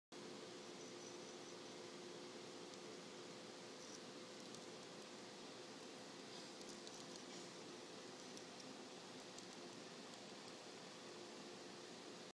Abacus magnetic watch
I just got this today and it's definitely the coolest watch I've had. The time is told by a little metal ball that rotates freely and can even be touched, but when you level out the watch the ball is caught by a hidden magnet to show the time.

PS. This is the first video I made and uploaded with my iPhone so I hope everything worked OK.